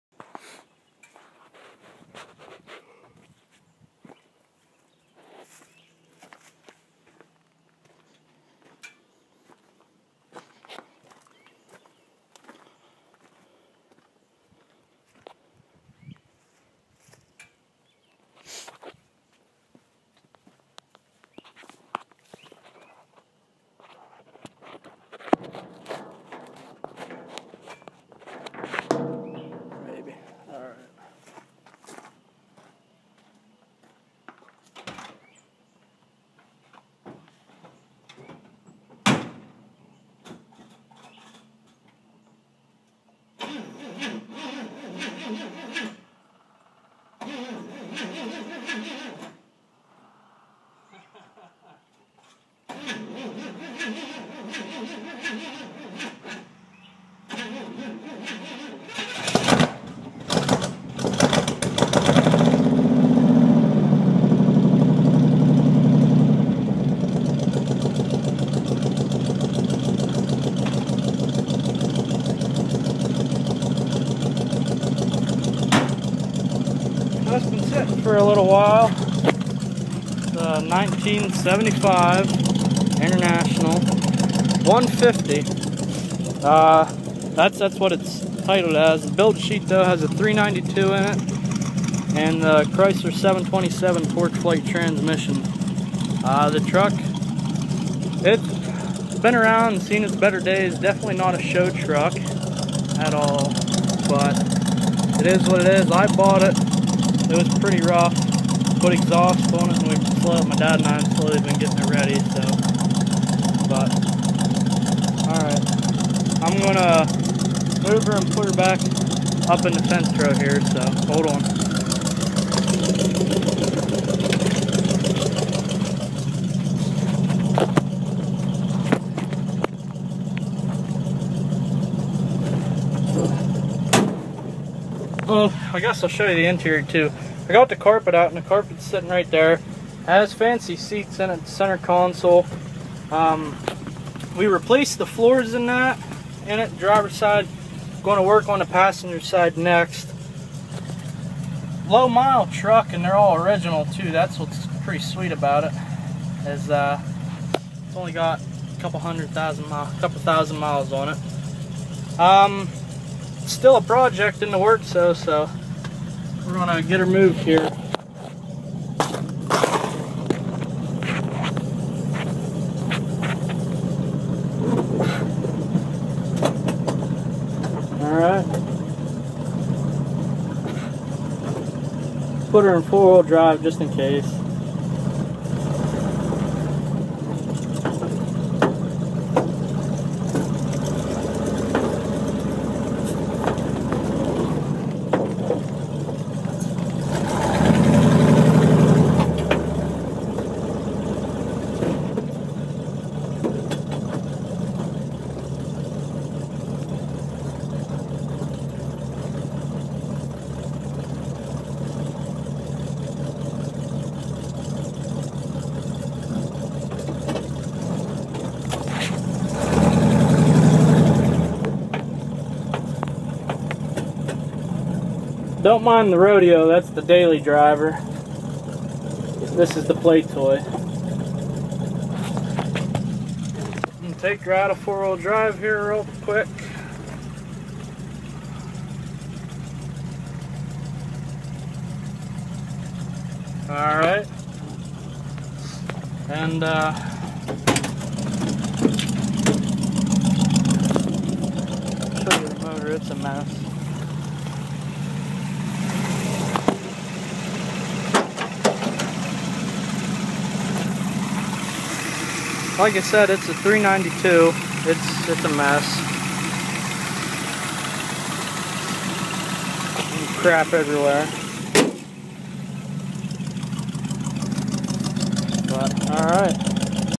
"Beatrice The Binder" 1975 International Harvester pickup
So I finally got around to making another video, this one is still pretty short 'cause I was in a hurry to get home. Basic intro to my baby, Beatrice the Binder (Internationals are known as Cornbinders) There is a good bit of history behind this old truck. She was special ordered back in the day by a man by the name of Ben Lingle for his hunting lodge in Shawville PA, known as Linglewood Lodge. Beatrice was part of a fleet of about eight or nine trucks: three pickups, two Travelalls two Travelettes, and I know of at least one Scout Terra possibly a Scout II as well. She has definitely had her better days, still in alright shape though, typical IH rust. Cool thing is, she has extremely low miles for the age, and most of those miles were all dirt. From what I've been told none of the trucks from Linglewood ever saw blacktop until the late eighties early nineties. Titled as a D150 half ton but ordered with most things from a one ton (392 V8 instead of 345, springs heavy enough to haul the average Walmart shopper, Dana 44 front and rears with factory limited slip lockers, etc...) Alright, so now you've got a quick look, I'm gonna try to put several more videos up about some of the work I've been doing to her. As of now she's about 85% street legal. (100% legal by my standards but still slightly frowned upon by local police). Until next time, keep your foot on the gas and your head outta your ass - BigBird